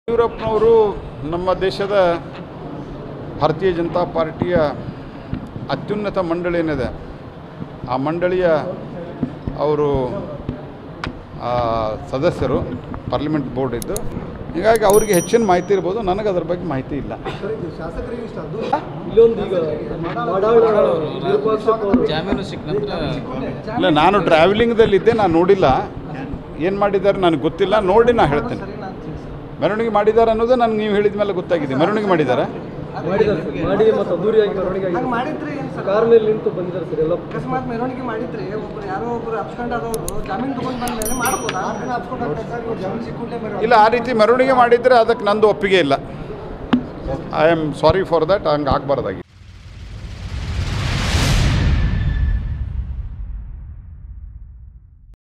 Pralhad Joshi: ಮಾಡಾಳ್​​​ ವಿರೂಪಾಕ್ಷಪ್ಪ ಮೆರವಣಿಗೆ ಮಾಡಿದ್ದಕ್ಕೆ ನನ್ನ ಒಪ್ಪಿಗೆ ಇಲ್ಲ! | Madal Virupashappa
In Dharwad Central Minister Pralhad Joshi tells he is not okay with Madal Virupakshappa's grand procession.